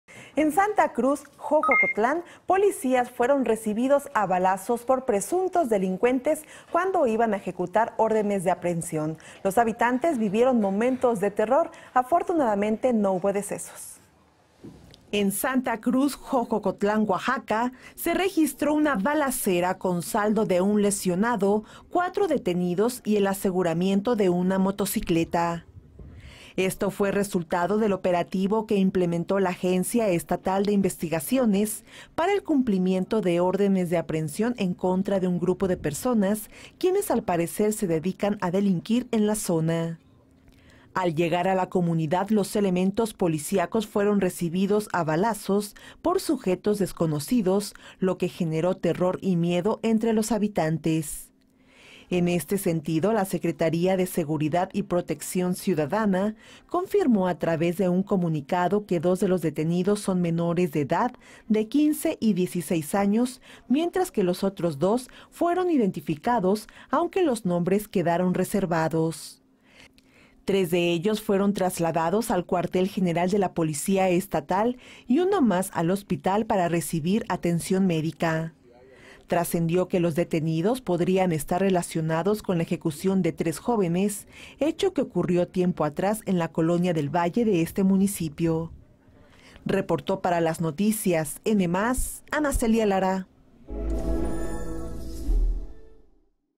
Balacera Causa Terror en Santa Cruz Xoxocotlán | Las Noticias Oaxaca -🔴 Noticias en vivo
Balacera | En Santa Cruz Xoxocotlán se dio un enfrentamiento a balazos entre policías y presuntos delincuentes, el saldo fue de un lesionado, cuatro detenidos y el aseguramiento de una motocicleta.

Con información de: 🎙️ Ana Celia Lara